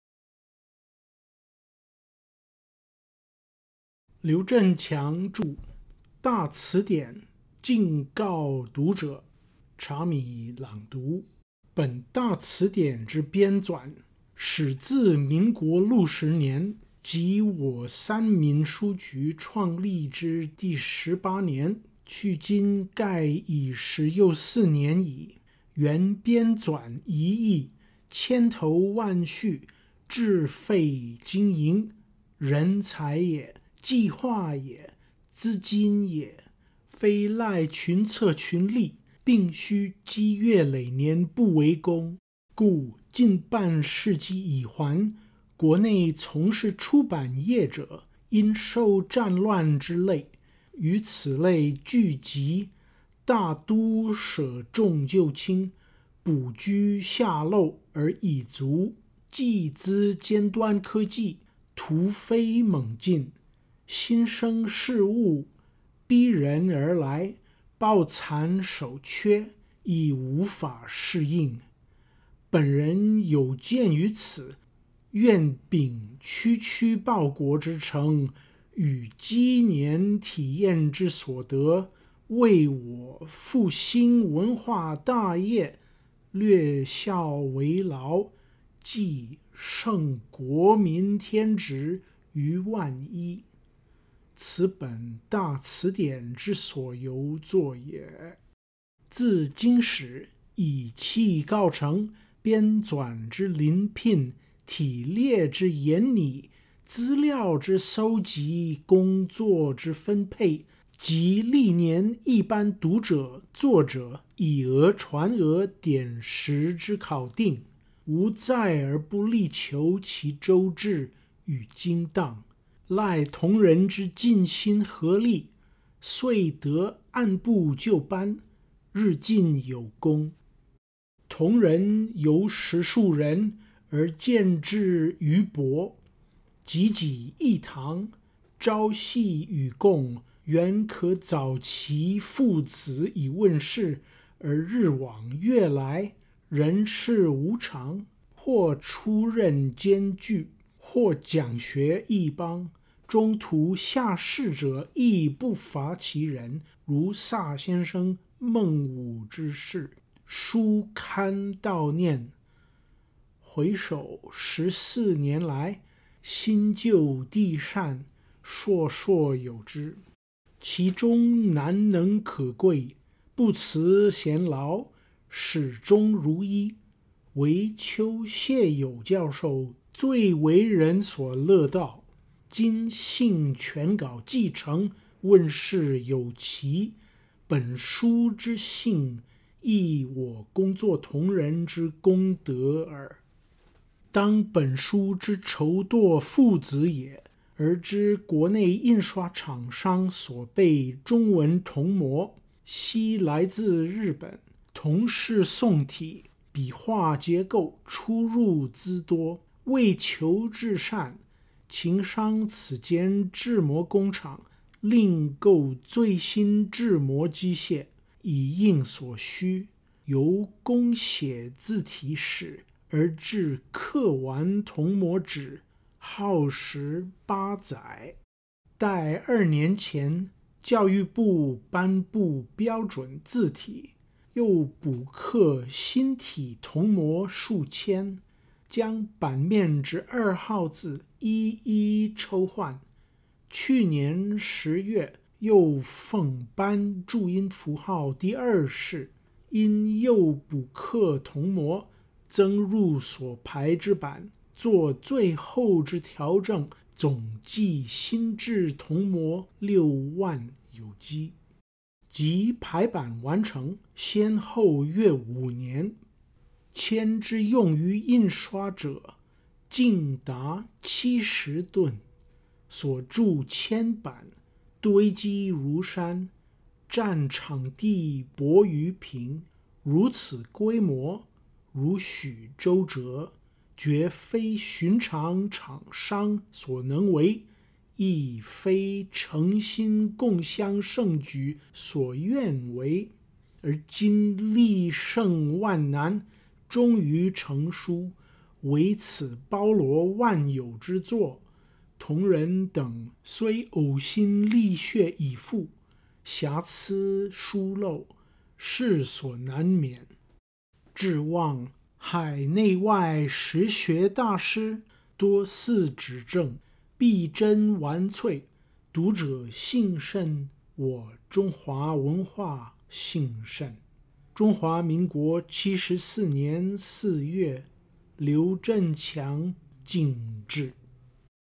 PLEASE SEE this video is not the newest version.
this video is not the newest version.

《大辭典》之前言「敬告讀者」，劉振強（1932年至2017）著，茶米　朗讀

"To Our Readers," preface to _Dà cídiǎn_ (1985), by Liú Zhènqiáng (1932–2017). Read by David Prager Branner.